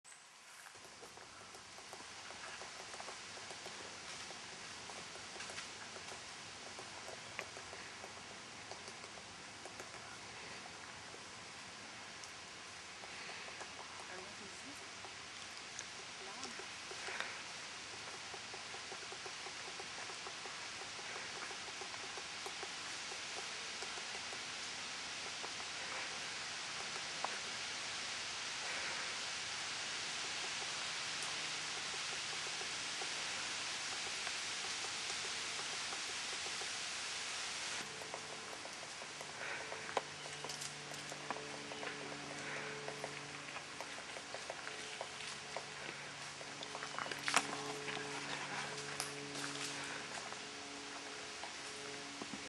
PIC BOIS-WOODPECKER
savage bird in the forest of Mont St Bruno Quebec
Pic-Bois en pleine action au Mont ST Bruno